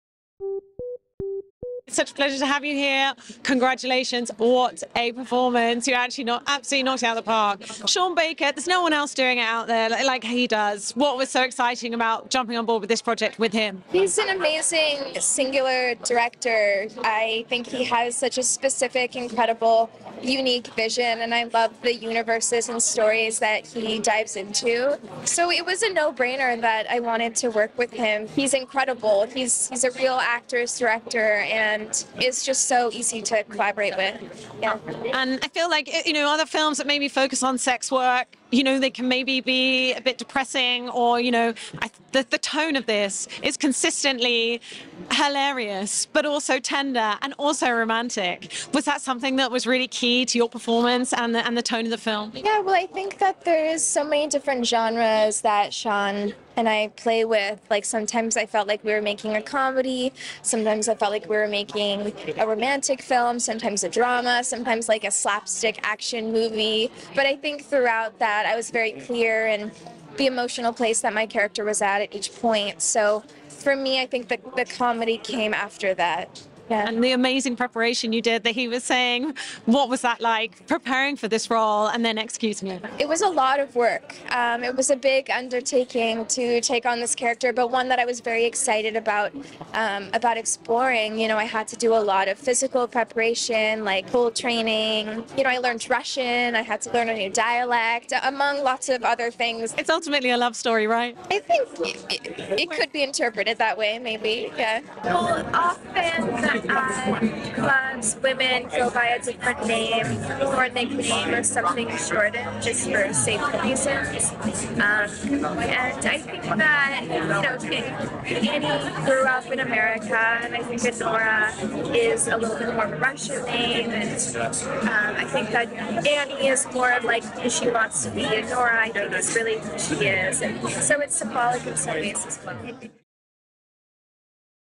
Mikey Madison interview on Anora at London Film Festival 2024: Comedy, romance & drama blend
Mikey Madison interview on Anora at London Film Festival 2024: Comedy, romance & drama blend, Sean Baker's unique vision & intense role prep

Mikey Madison discusses her exhilarating experience working with Sean Baker on Anora at the London Film Festival 2024. Madison praises Baker's unique vision and the seamless collaboration, touching on the film's blend of comedy, romance, and drama. She talks about the extensive preparation for her role, from pool training to learning Russian, highlighting the film's exploration of identity and love. Madison's insights provide a tantalising glimpse into the making of Anora, a testament to innovative storytelling and character depth.